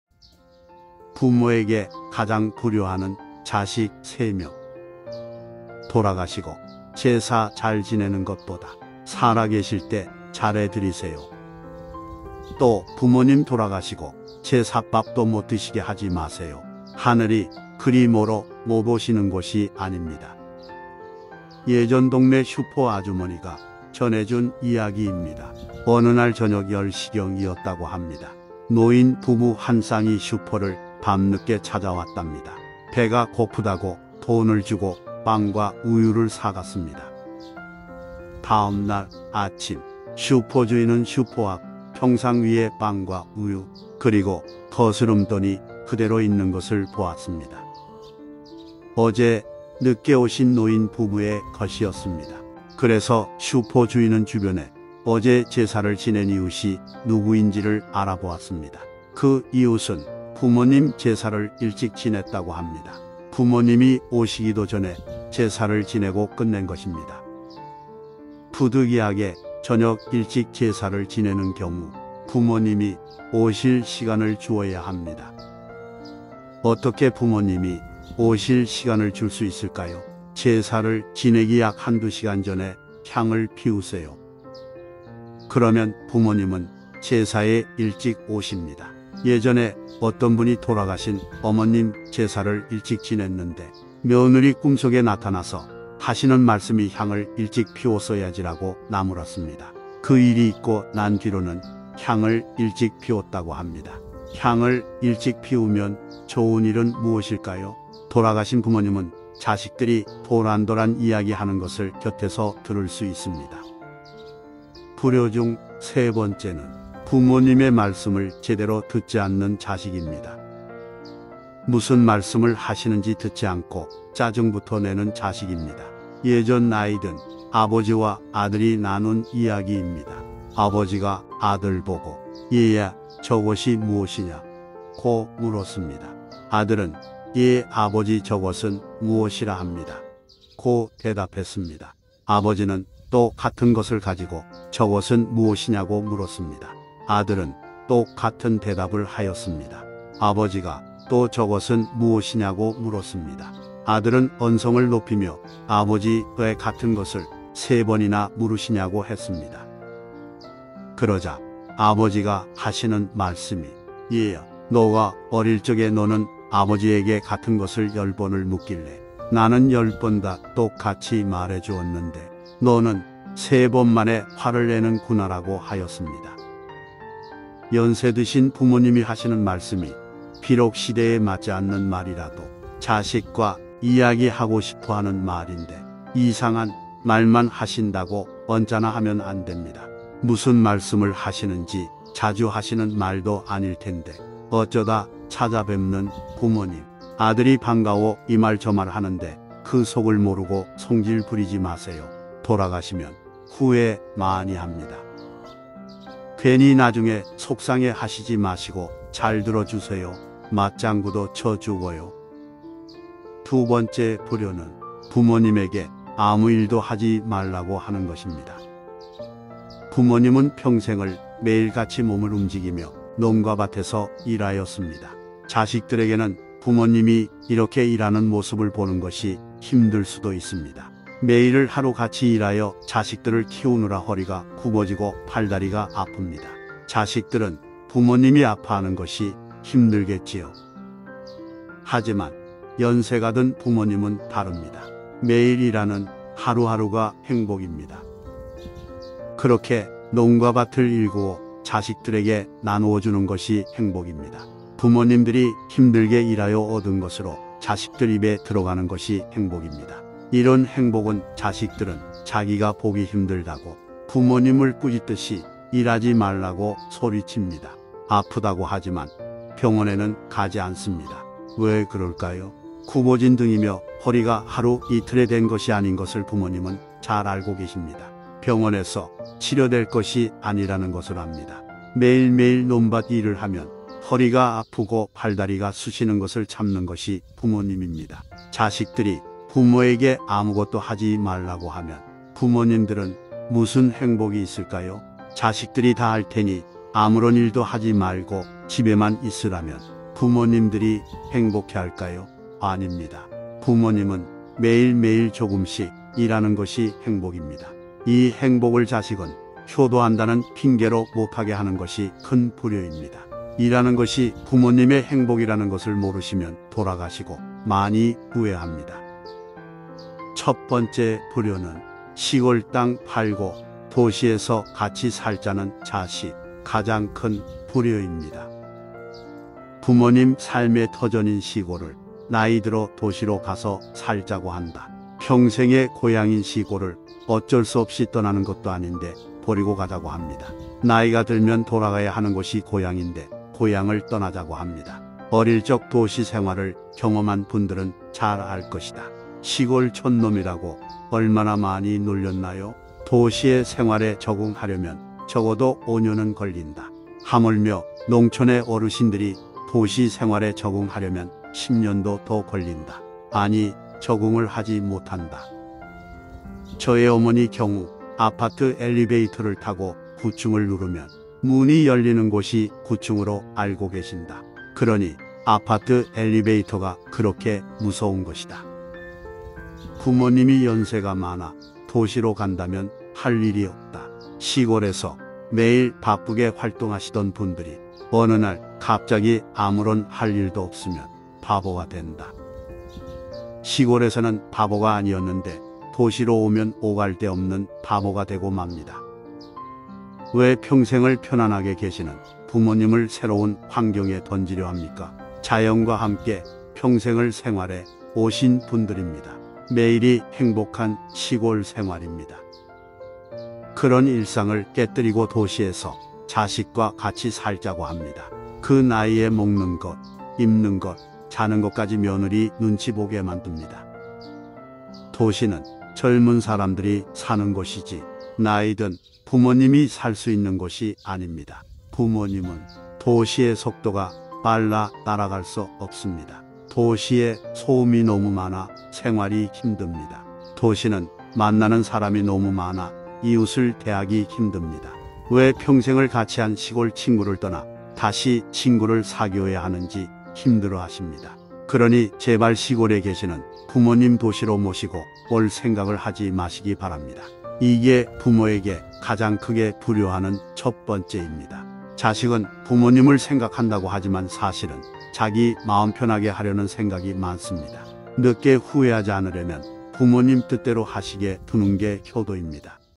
부모에게 가장 불효하는 자식 3명, 부모님께 효도하는 방법, 불효와 효도 방법
이렇게 하시면 안됩니다.
부모님에게 효도하는 방법 별것 아닙니다.